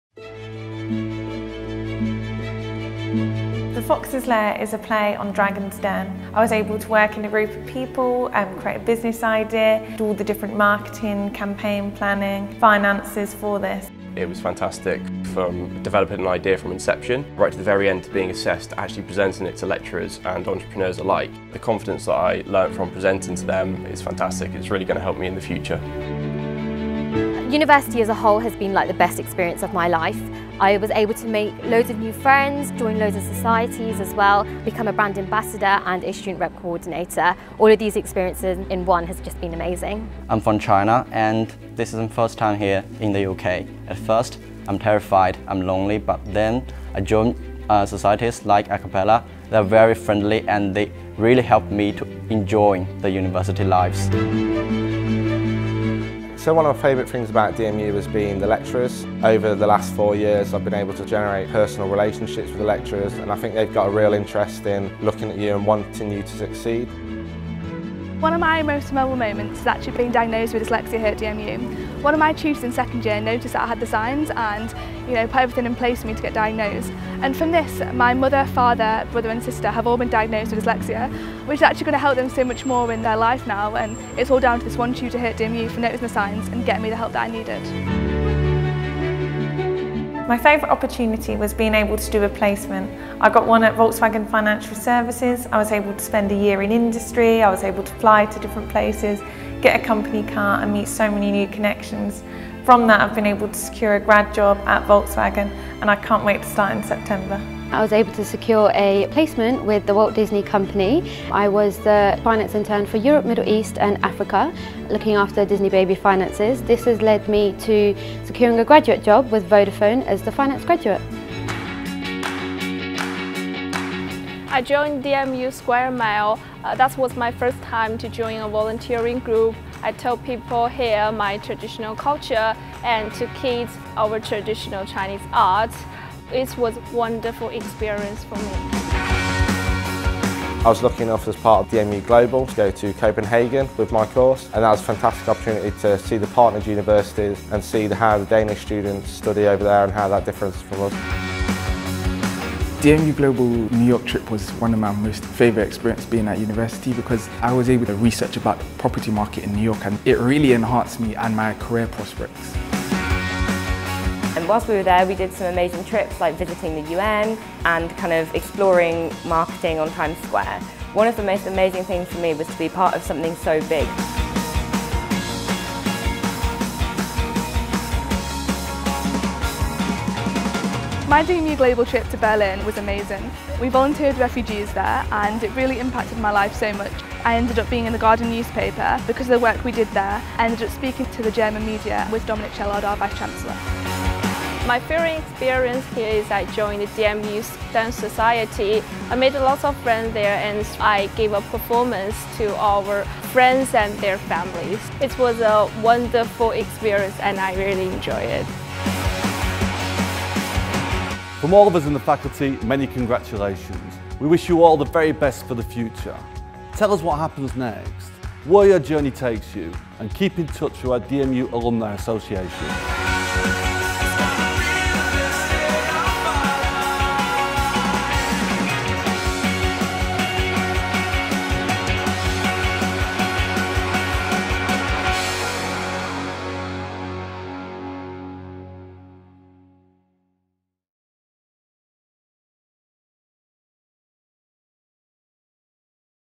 CLASS OF 2017: Faculty of Business and Law highlights
We pay tribute to the Faculty of Business and Law Class of 2017 in this video first shown at our DMUgrads ceremonies.